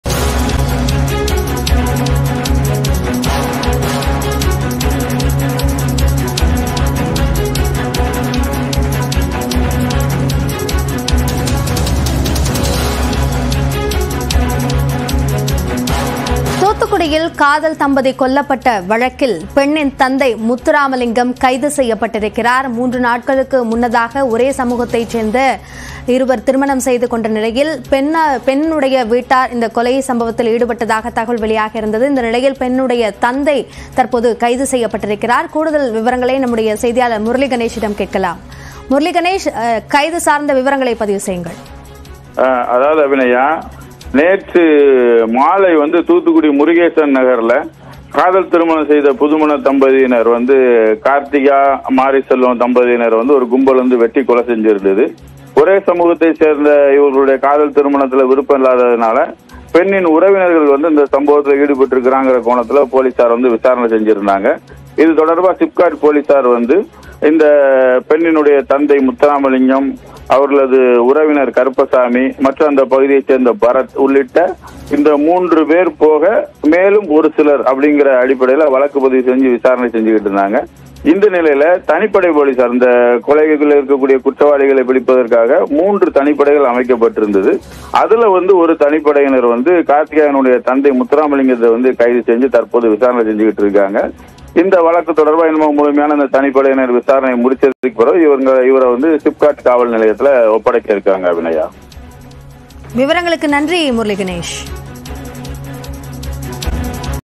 காதல் தம்பதி கொடூர கொலை - பெண்ணின் தந்தை கைது | Love Marriage | Thoothukudi | Shocking News
தூத்துக்குடியில் காதல் தம்பதி கொல்லப்பட்ட வழக்கில் பெண்ணின் தந்தை முத்துராமலிங்கம் கைது.